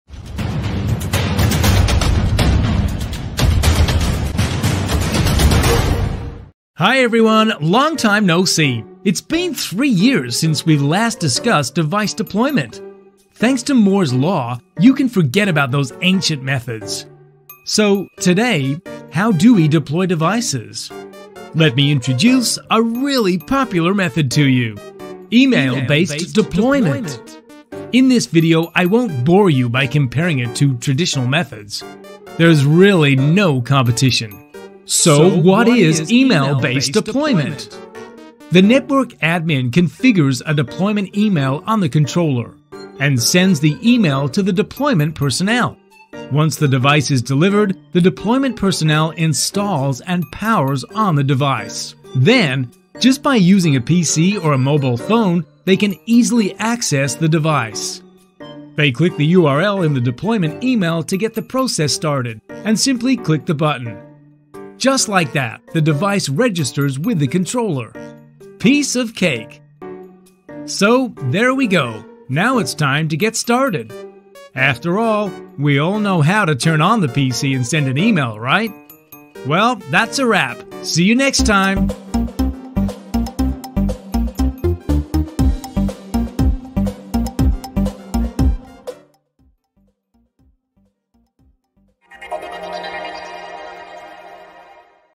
Huawei SD-WAN Solution ：What is Email-based Deployment?
What can you do in 1 minute? Blink 12 times? Now, spend just 1 minute to watch what the email-based deployment feature of Huawei SD-WAN can do. Don't blink, or you'll miss it. NetworkPulse SD-WAN